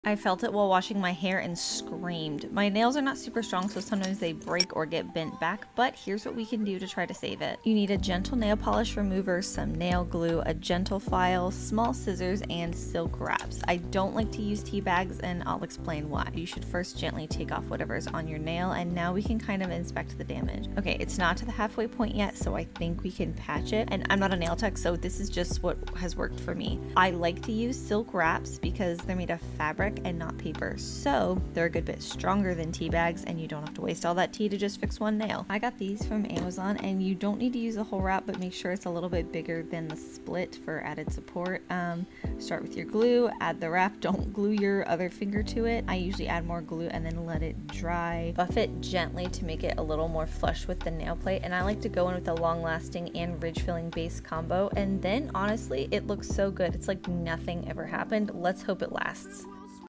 how to fix a broken nail 😩🔪
HOW TO FIX A BROKEN NAIL - this is just one of the methods you can use and I'm not a nail tech so this what I know. I've used the teabag method for nails before and it works temporarily, but using silk wraps is better and tends to last longer! You can also use an extension or fake nail to cover the break and I can cover that in another video if this one hits 10,000 likes:)

Nail Polish Remover Pot
Nail Glue
Nail Scissors
Silk Wraps

💅 Amazon nail favs (affil

🖌️ Nail Art Brushes (aff)

💧Drip Ring (code thegraytergood) (aff)